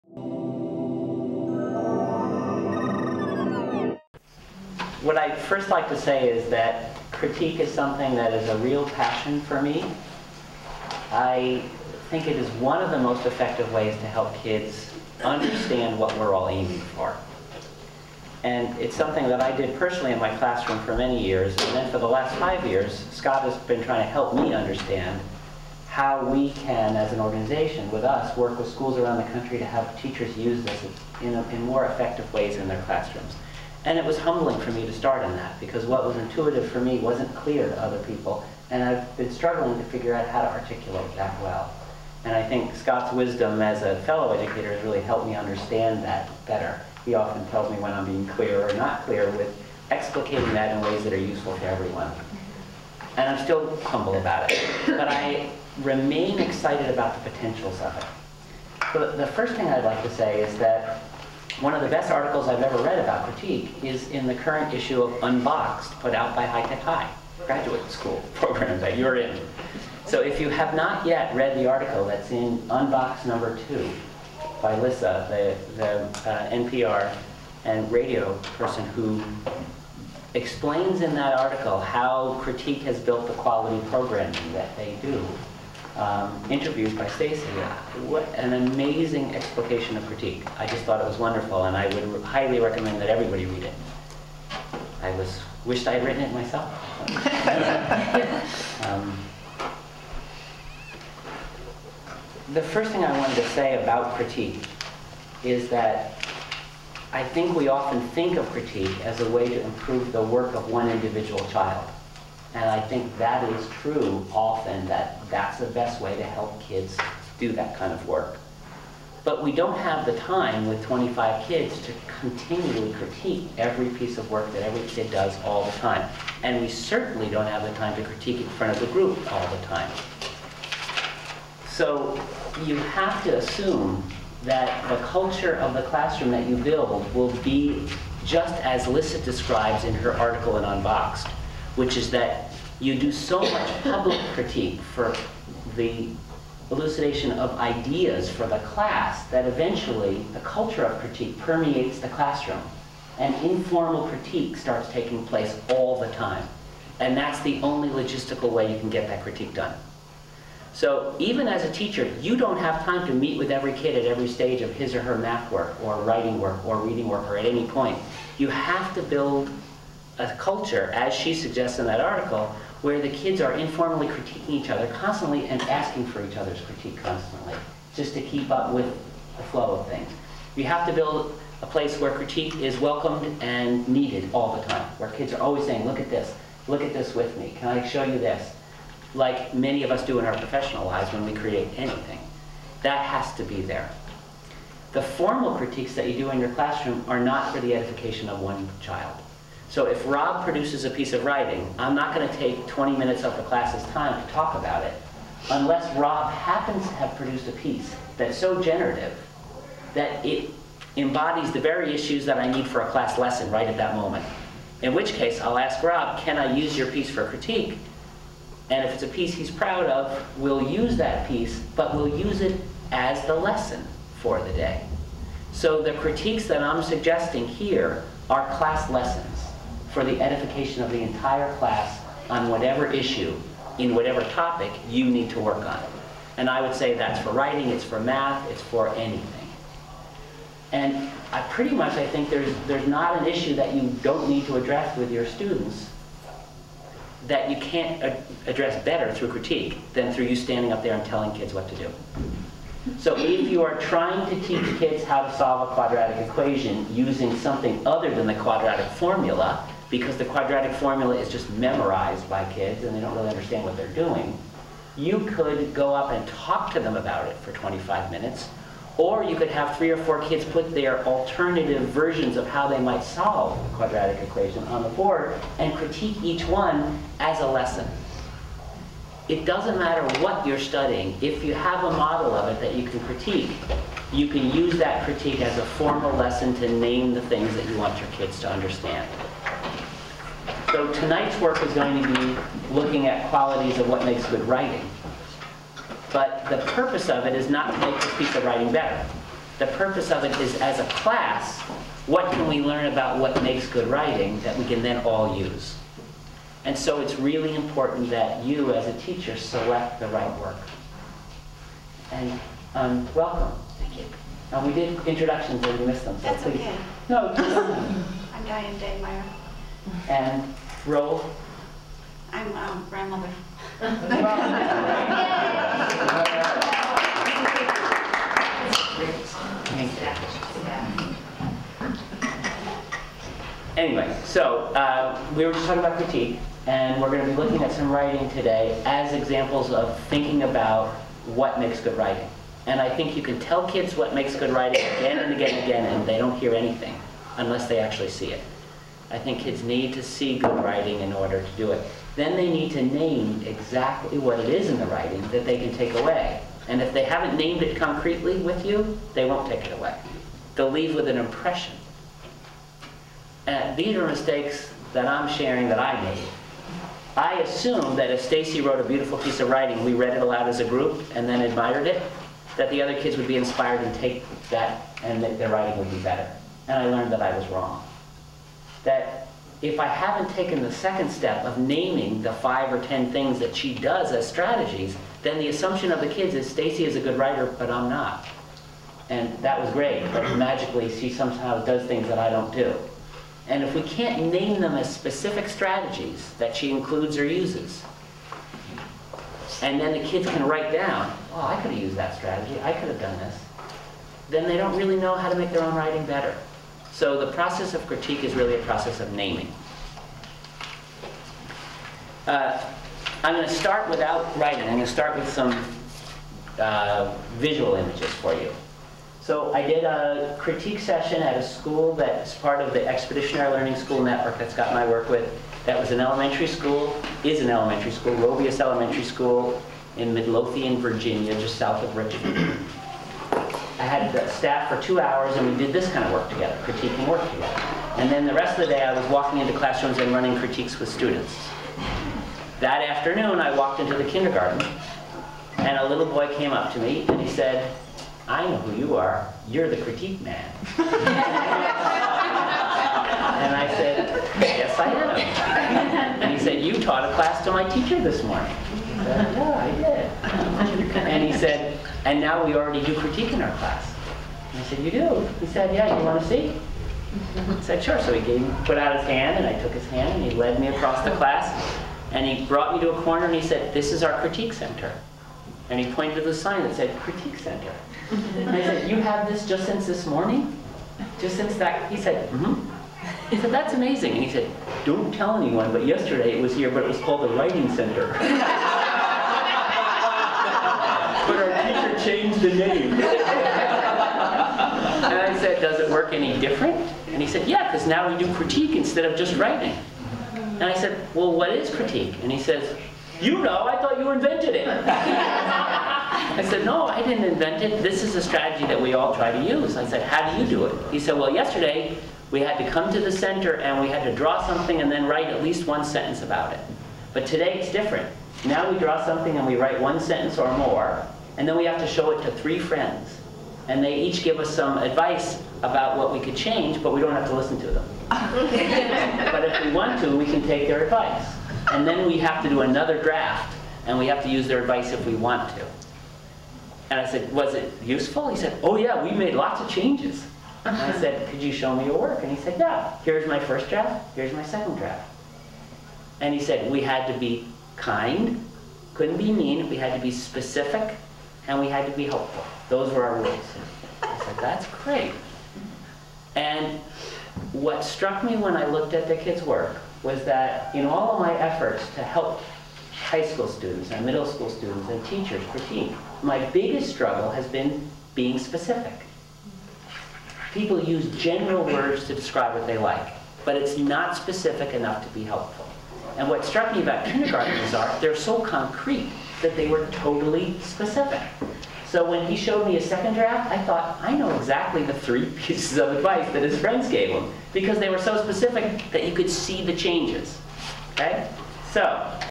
Ron Berger on Critique (1 of 2)
Master teacher Ron Berger of Expeditionary Learning discusses revision and critique, with examples from the work of first graders.